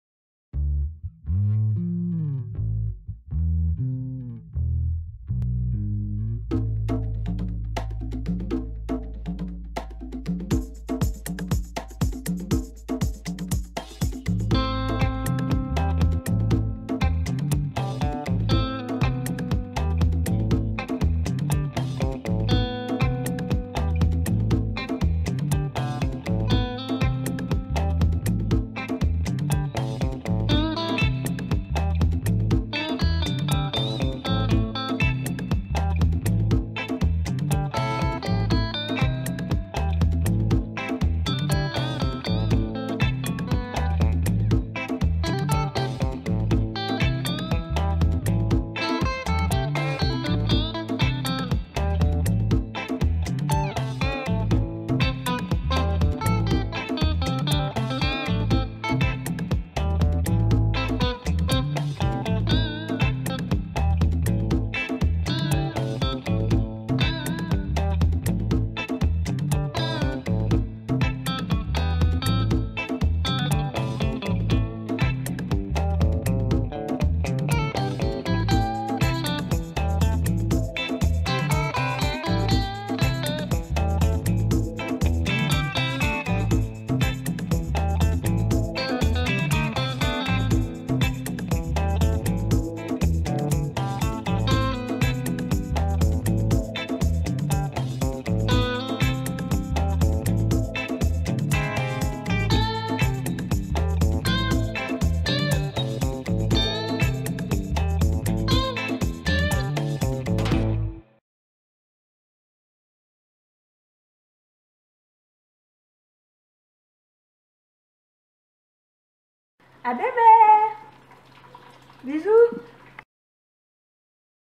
La production du Bissap
Vidéo sur la production de jus de Bissap (à base de fleurs sechées d'hibiscus) par Florence Sawadogo de Faso'Die